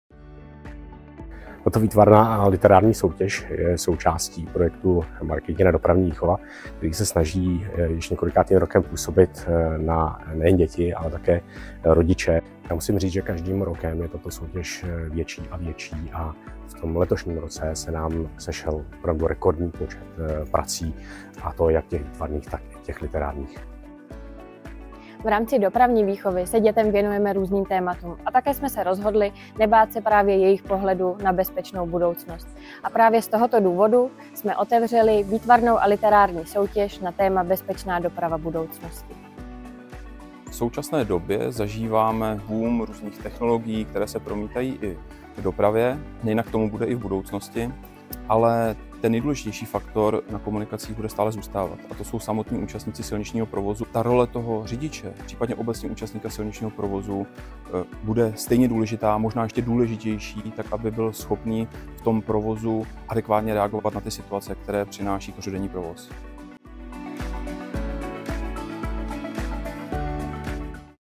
Markétina dopravní výchova - Vyhlášení soutěže Bezpečná doprava budoucnosti
V pondělí 24. června proběhlo ocenění nejlepší výtvarníků a literátů, dětí, které se zapojili do soutěže Bezpečná doprava budoucnosti, kterou jsme vyhlásili ve spolupráci s Policie České republiky.

Vyhlášení se konalo v jedinečných prostorech Kampusu ČSOB  za účasti významných hostů.🩷💛🩵 Děkujeme, že děti mohly převzít ocenění z rukou Evy Pavlové, manželky prezidenta ČR, genpor. Martina Vondráška, policejního prezidenta, Aleše Blažka, CEO ČSOB a našeho Jana Poláka, ředitele Týmu.😊